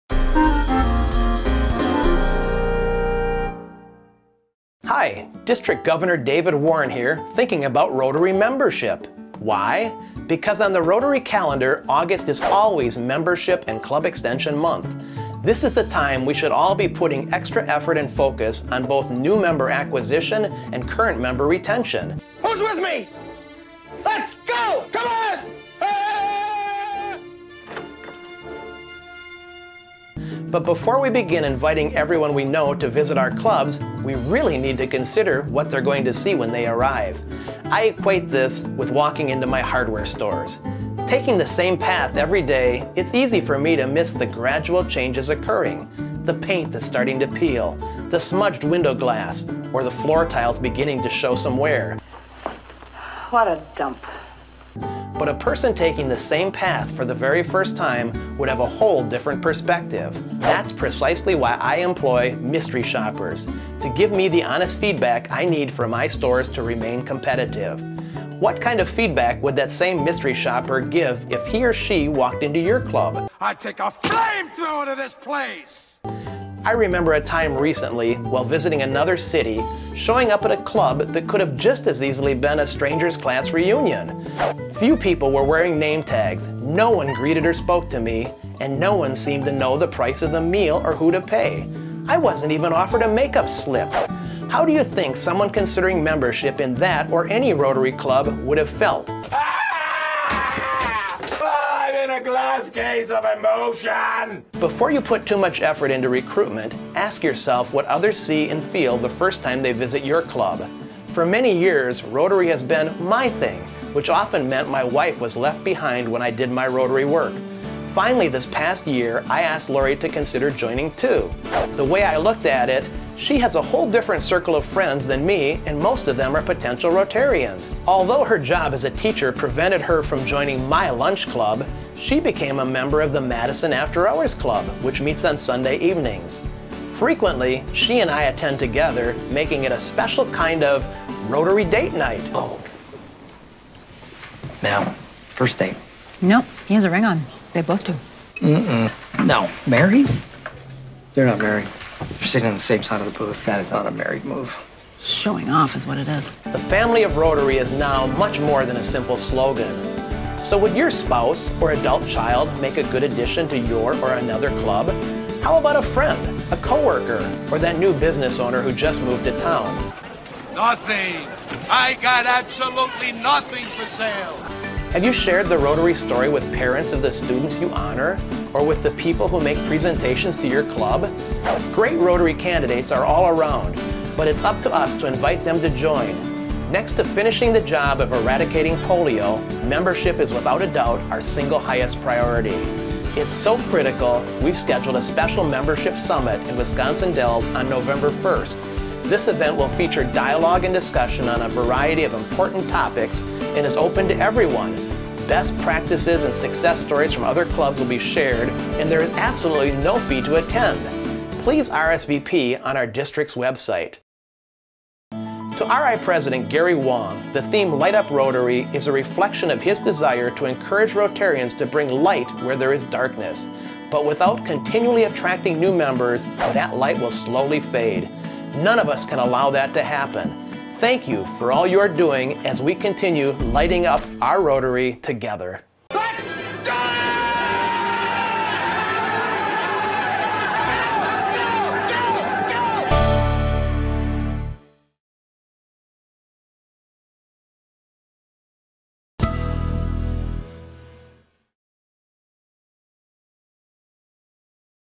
Membership - Rotary International District 6250 - Video Newsletter August 2014
District 6250 Governor David Warren shares some thoughts about increasing and retaining membership in our Rotary clubs.